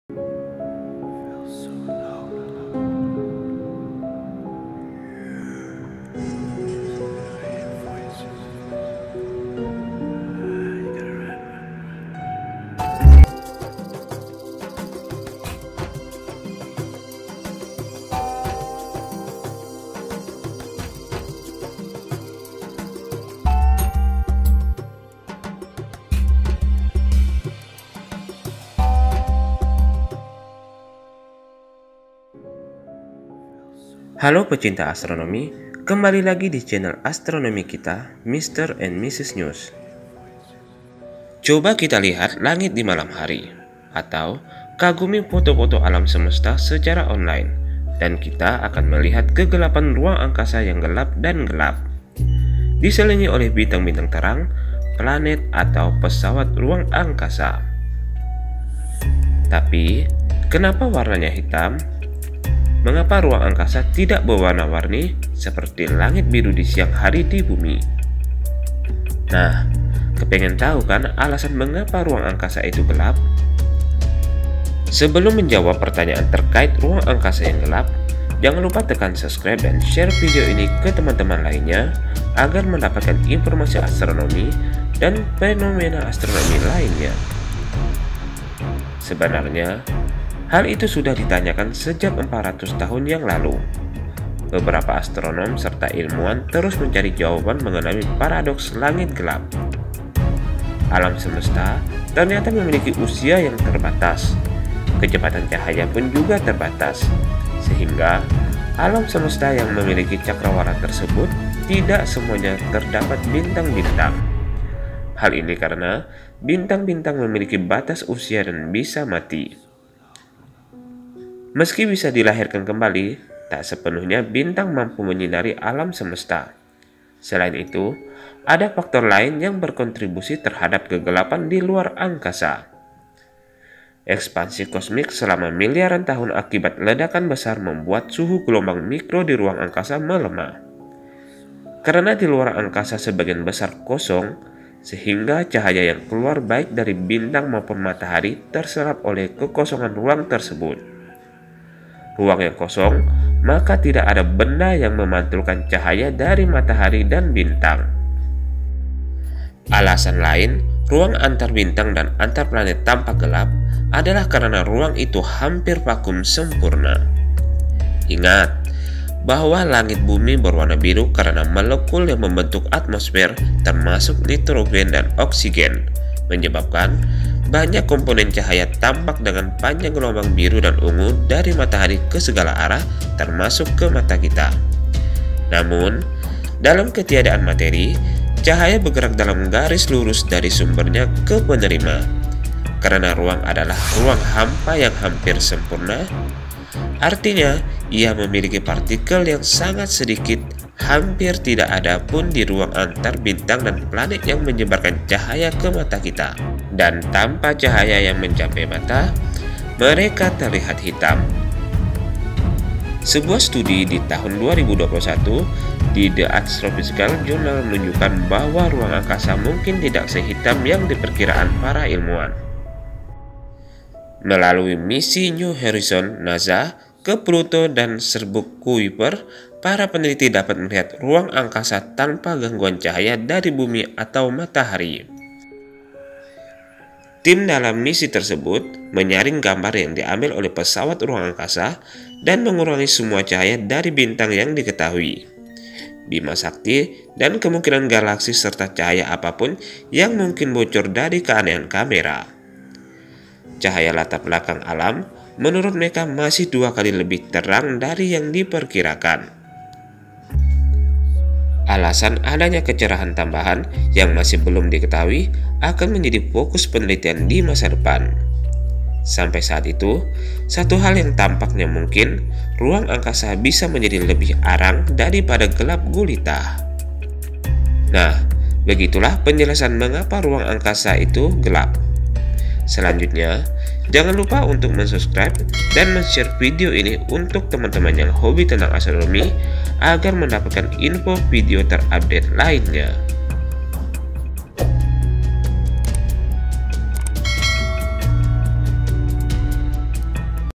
KENAPA RUANG ANGKASA GELAP ? INI ALASANNYA !! ( why is space dark )
Terimakasih !!

Sebenarnya  hal itu juga sudah ditanyakan sejak 400 tahun yang lalu. Beberapa astonom serta ilmuwan terus mencari jawaban mengenai paradoks langit gelap. Alam semesta ternyata memiliki usia yang terbatas. Kecepatan cahaya pun juga terbatas. Sehingga alam semesta yang memiliki cakrawala tersebut tidak semuanya terdapat bintang-bintang. Hal ini karena bintang-bintang memiliki batas usia dan bisa mati.Meski bisa dilahirkan kembali, tak sepenuhnya bintang mampu menyinari alam semesta. Selain itu ada faktor lain yang berkontribusi terhadap kegelapan di luar angkasa. Ekspansi kosmik selama miliaran tahun akibat ledakan besar membuat suhu gelombang mikro di ruang angkasa melemah. Karena di luar angkasa sebagian besar kosong sehingga cahaya yang keluar baik dari bintang maupun matahari terserap oleh kekosongan ruang tersebut. Ruang yang kosong maka tidak ada benda yang memantulkan cahaya dari matahari dan bintang.Alasan lain ruang antarbintang dan antarplanet tampak gelap adalah karena ruang itu hampir vakum sempurna. Ingat, bahwa langit bumi berwarna biru karena molekul yang membentuk atmosfer, termasuk nitrogen dan oksigen, menyebarkan banyak komponen cahaya tampak dengan panjang gelombang biru dan ungu dari matahari ke segala arah, termasuk ke mata kita. Namun, dalam ketiadaan materi, cahaya bergerak dalam garis lurus dari sumbernya ke penerima. Karena ruang adalah ruang hampa yang hampir sempurna — artinya ia memiliki partikel yang sangat sedikit — hampir tidak ada apa pun di ruang antara bintang dan planet yang menyebarkan cahaya ke mata kita. Dan tanpa cahaya yang mencapai mata, mereka terlihat hitam.Konon, sebuah studi tahun 2021 di The Astrophysical Journal menunjukkan bahwa ruang angkasa mungkin tidak sehitam yang diperkirakan para ilmuwan. Melalui misi New Horizons NASA ke Pluto dan Sabuk Kuiper, para peneliti dapat melihat ruang angkasa tanpa gangguan cahaya dari Bumi atau matahari. Tim menyaring gambar yang diambil oleh pesawat ruang angkasa dan mengurangi semua cahaya dari bintang yang diketahui, Bima Sakti dan kemungkinan galaksi, serta cahaya apa pun yang mungkin bocor dari keanehan kamera. Cahaya latar belakang alam semesta, menurut mereka, masih dua kali lebih terang dari yang diperkirakan.